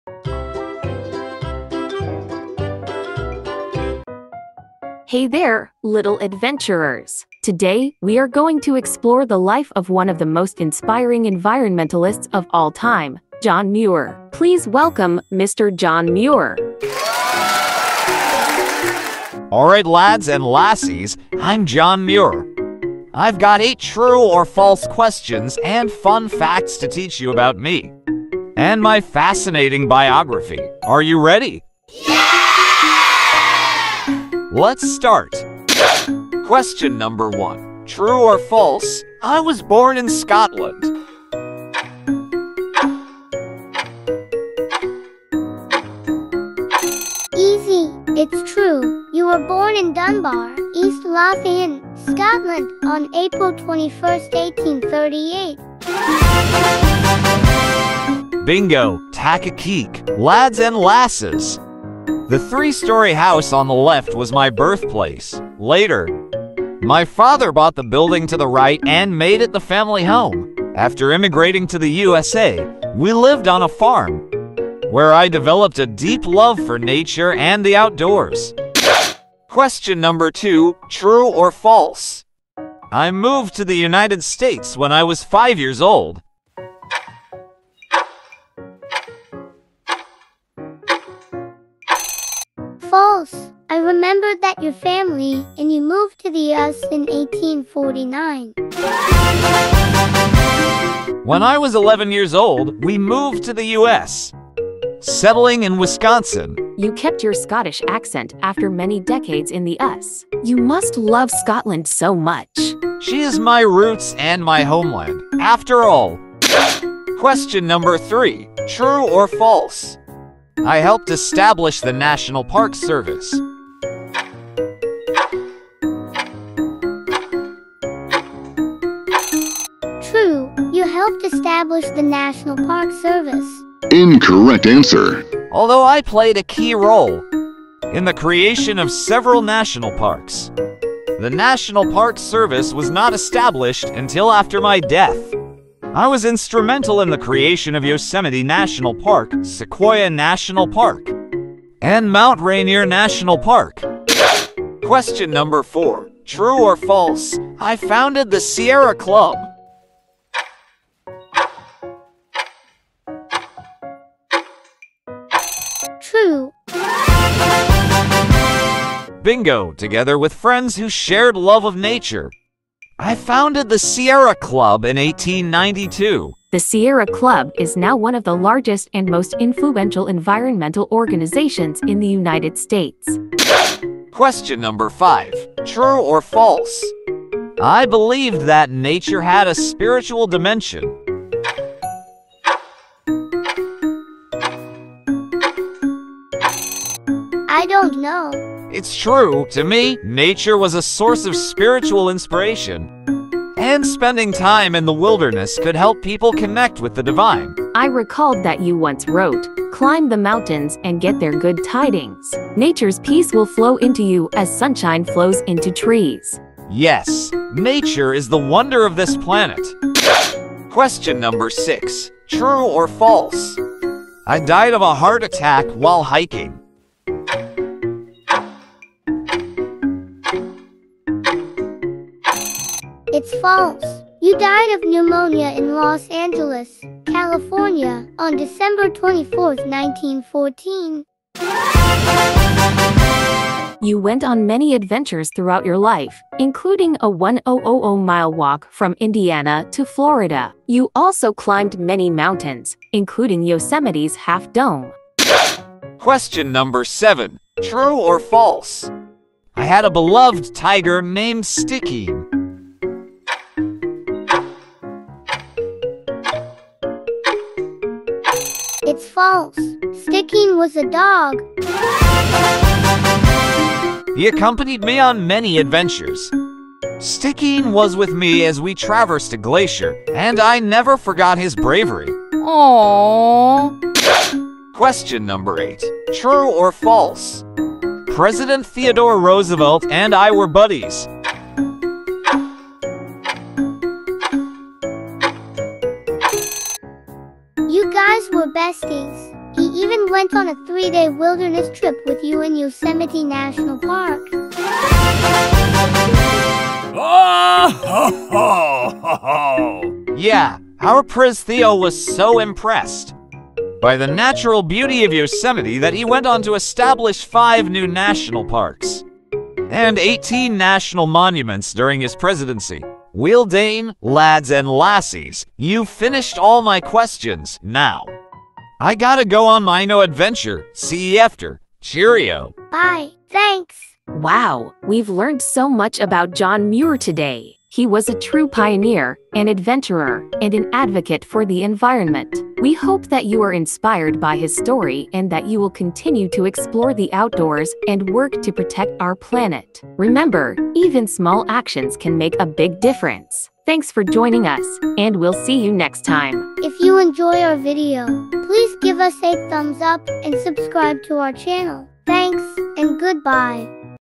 John Muir for Kids: Discover His Amazing Adventures and Biography
If you're looking for an exciting and educational video about one of the most important figures in American environmental history, then you won't want to miss "John Muir for Kids: Discover His Amazing Adventures and Biography!"

In this video, we'll take you on a journey through the incredible life of John Muir, from his early years as a shepherd in Scotland, to his incredible adventures exploring the American wilderness, to his tireless efforts to preserve our country's natural wonders for future generations.

Along the way, we'll share some of the most fascinating and fun facts about John Muir's life, as well as some surprising true or false questions to test your knowledge.

With stunning visuals and engaging narration, "John Muir for Kids" is the perfect video for young learners who want to discover more about this inspiring and influential figure in American history. So sit back, relax, and get ready to be amazed by the adventures and biography of John Muir!